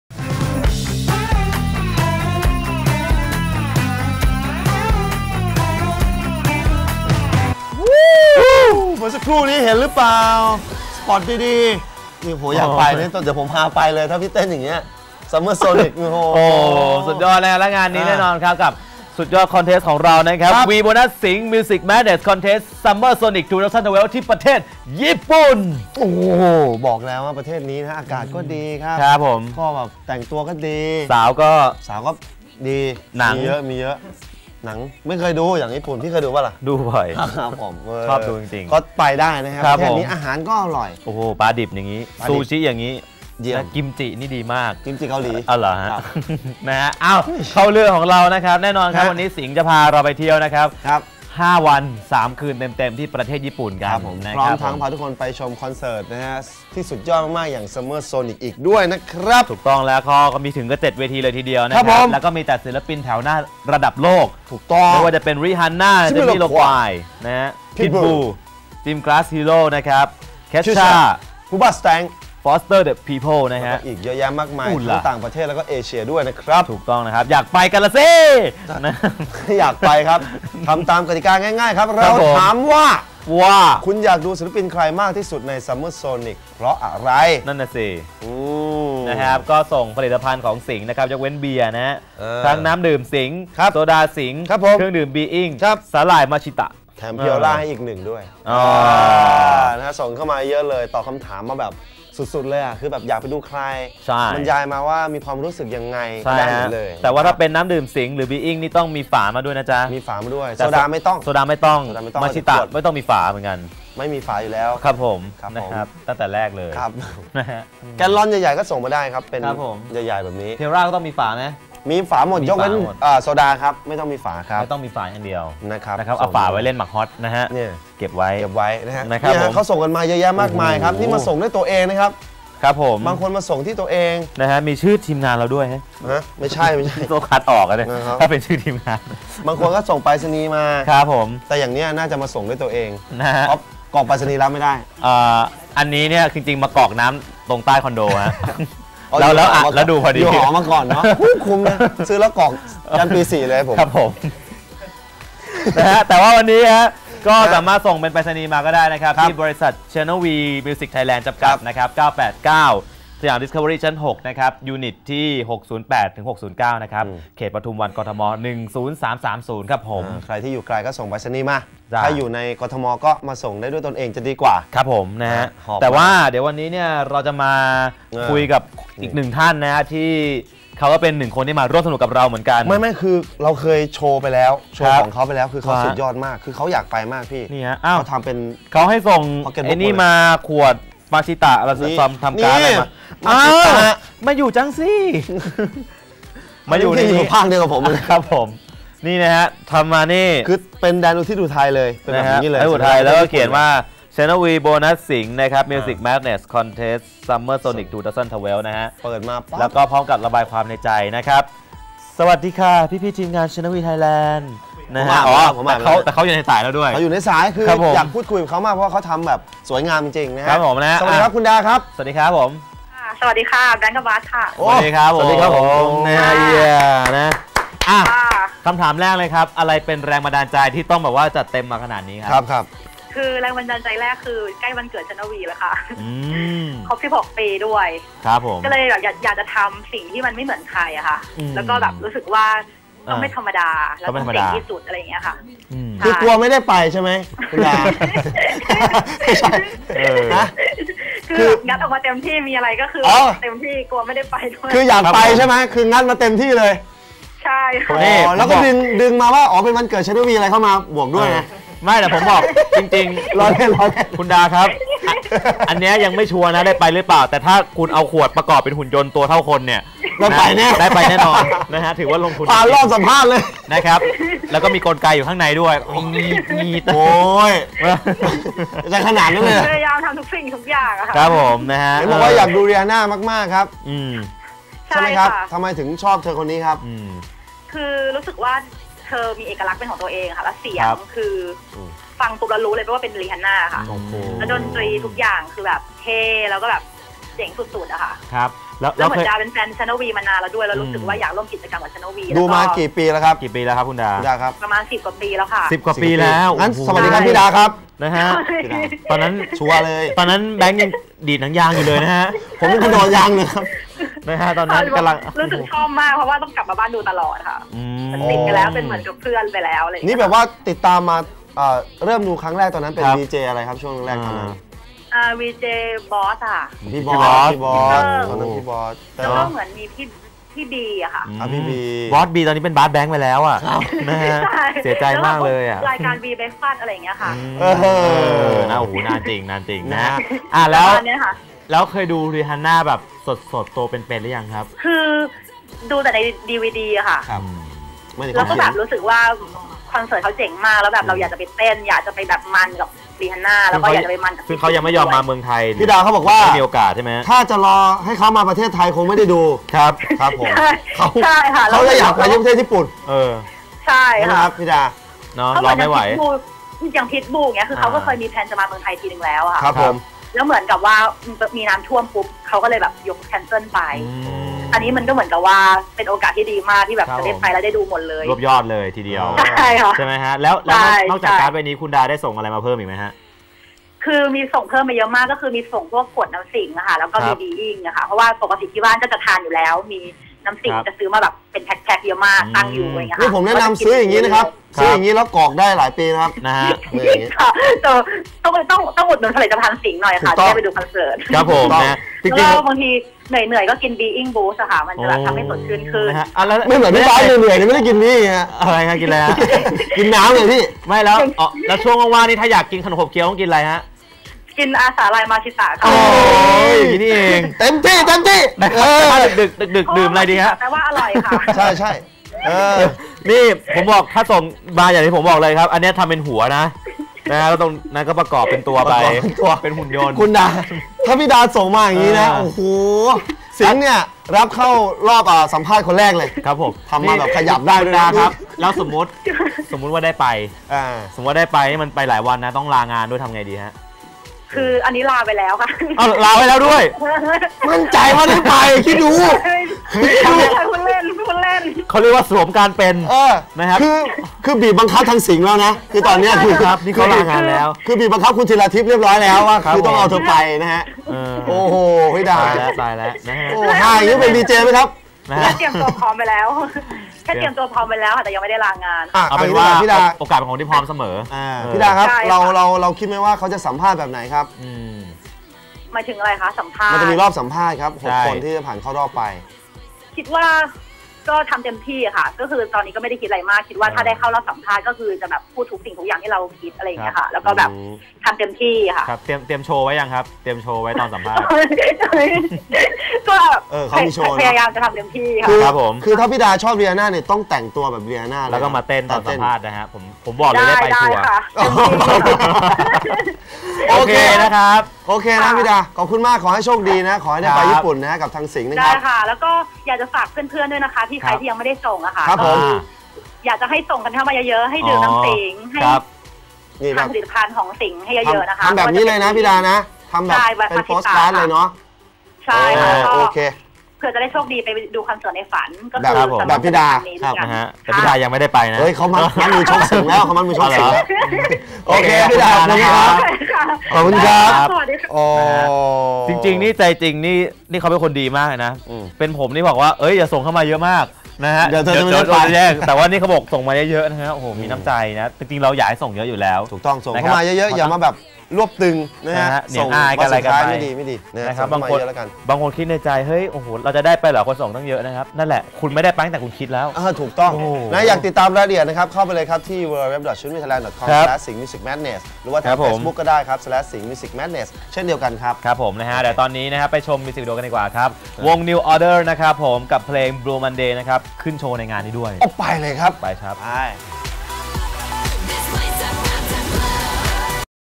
[V] Volunteer วันที่ 18 ก.ค. 55 - Summer sonic 2012
[v] Bonus Singha Music Madness Contest " summer sonic 2012"

ถ้าคุณคลั่งไคล้ประเทศญี่ปุ่นคลั่งไคล้ในการดูคอนเสิร์ต คราวนี้เตรียมเพิ่มดีกรีความคลั่งไคล้ให้ถึงขีดสุด เมื่อสิงห์ คอร์ปอเรชั่น พาคุณไปเที่ยวญี่ปุ่นและดูคอนเสิร์ตแบบฟรี ฟรี! 5 วัน 3 คืน ถึง 6 คน กับ [v] Bonus Singha Music Madness Contest " summer sonic 2012"
เที่ยวตะลุยโตเกียวแบบสุดโต่ง และสัมผัสกับเทศกาลดนตรีระดับโลกครั้งยิ่งใหญ่

รายละเอียดเพิ่มเติมที่